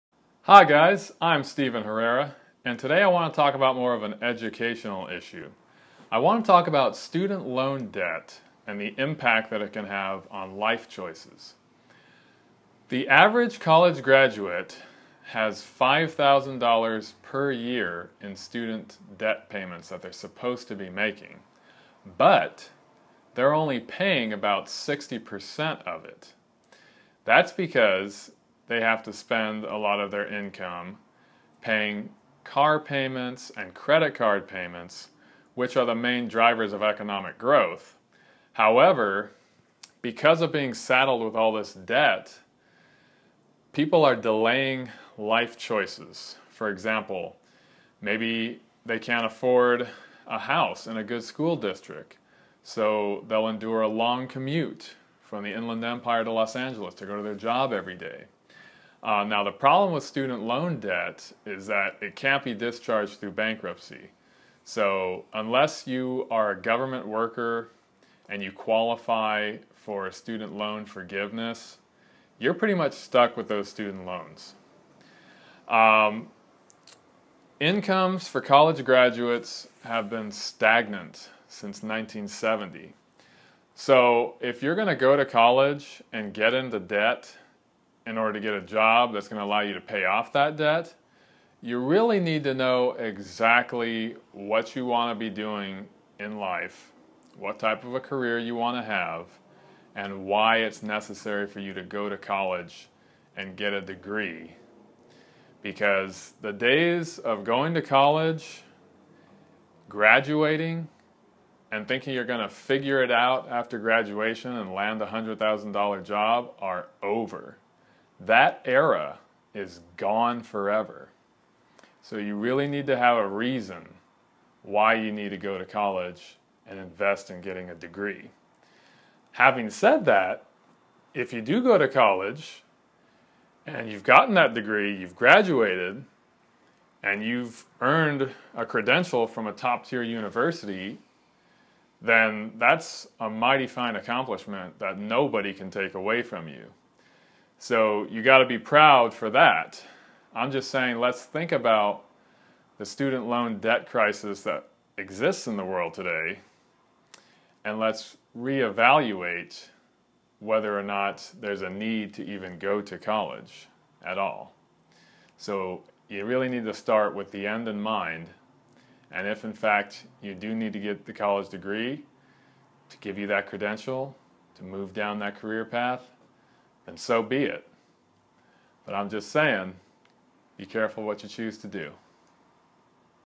Student Loan Debt Impacts Life Choices
In this video, I talk about how student loan debt can impact life choices.